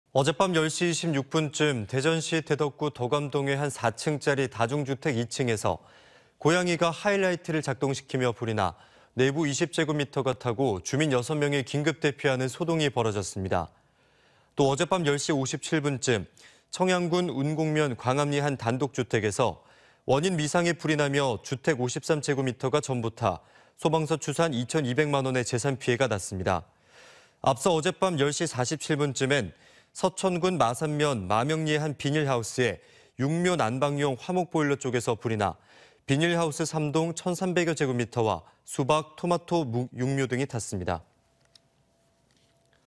대전과 청양 주택·서천 비닐하우스서 화재 잇따라｜ TJB 대전·세종·충남뉴스
어젯밤 10시 26분쯤
대전시 대덕구 덕암동의
한 4층짜리 다중주택 2층에서
고양이가 하이라이트를
작동시키며 불이 나
내부 20제곱미터가 타고
주민 6명이 긴급대피하는
소동이 벌어졌습니다.

또 어젯밤 10시 57분쯤
청양군 운곡면 광암리의
한 단독주택에서
원인 미상의 불이 나며
주택 53제곱미터가 전부 타
소방서 추산 2천 2백만 원의
재산 피해가 났습니다.

앞서 어젯밤 10시 47분쯤엔
서천군 마산면 마명리의
한 비닐하우스의
육묘 난방용
화목보일러 쪽에서 불이 나
비닐하우스 3동 1천 3백여 제곱미터와
수박, 토마토 육묘 등이 탔습니다.